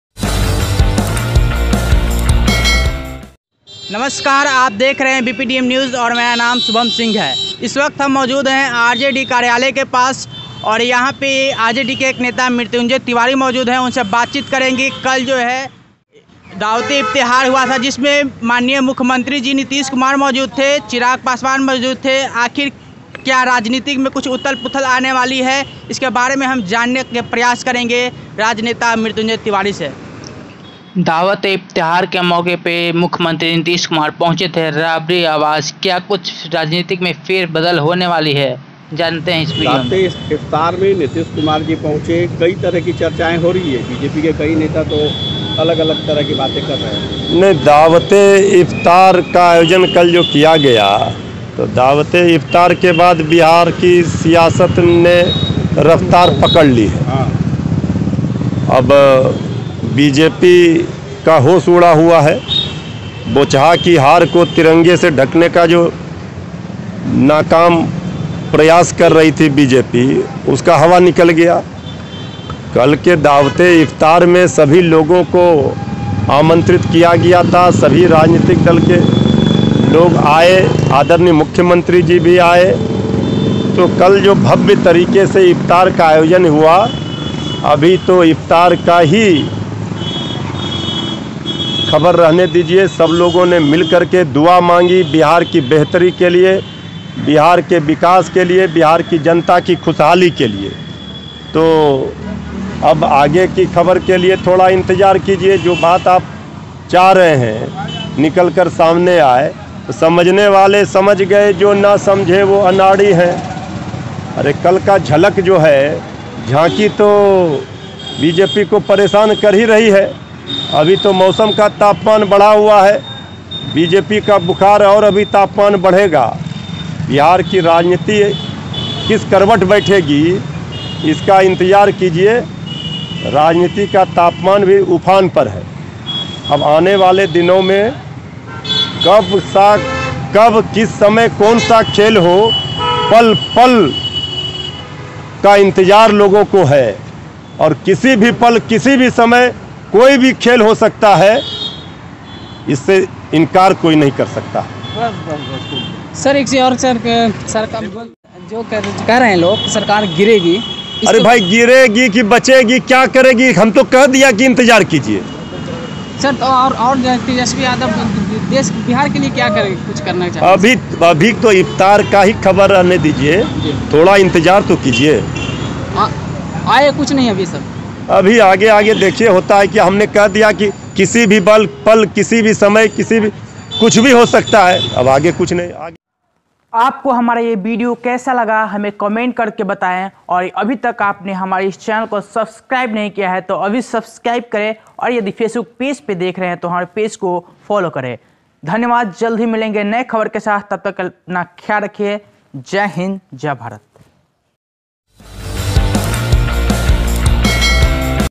बिहार के सत्ता में जल्द होने जा रही है फेरबदल | राजद नेता Mritunjay tiwari ने बताया सच latest news
बिहार में सत्ता में जल्द होने जा रही है फेरबदल | राजद नेता Mritunjay tiwari ने बताया सच  latest news